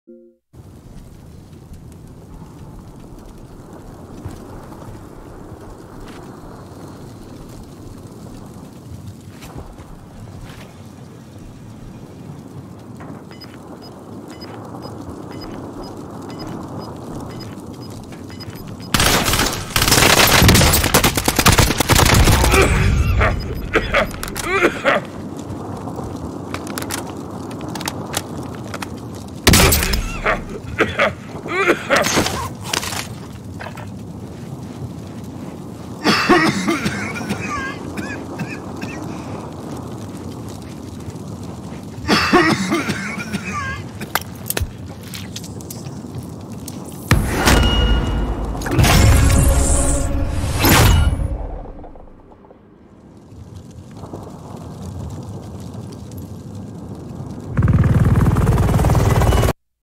What the What??? 😁🤞👌👀
The match lasted all the way to the Final circle ,as FJ clutched another for LLG.